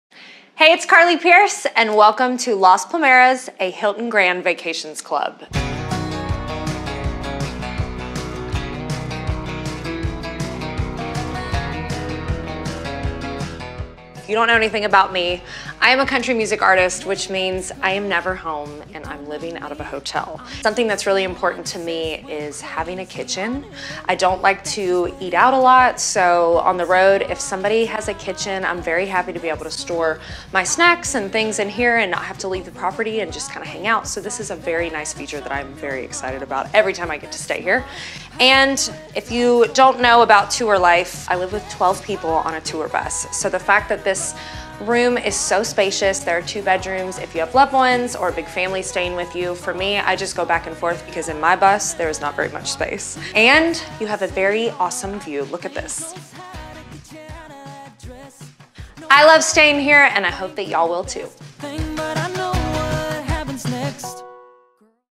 Tour a Suite with Carly Pearce at Las Palmeras, a Hilton Grand Vacations Club
Wondering where to stay in Orlando, Florida? Join Carly Pearce on a Suite tour at Las Palmeras, a Hilton Grand Vacations Club, and see why the country music star loves to stay with HGV. Enjoy spacious accommodations, an in-suite kitchen and everything you need on your next getaway.